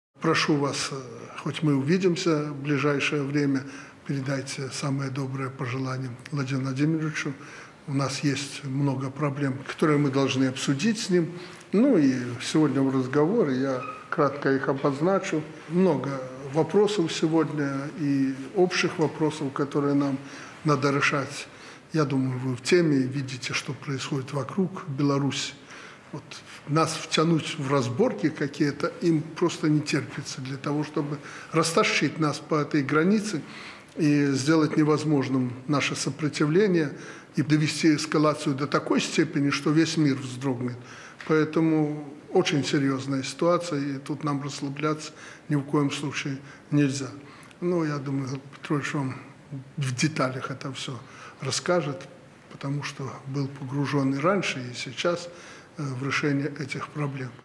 Лукашенко заявил о многих проблемах, которые хочет обсудить с Путиным
Видео: "Пул Первого". Читайте подробности на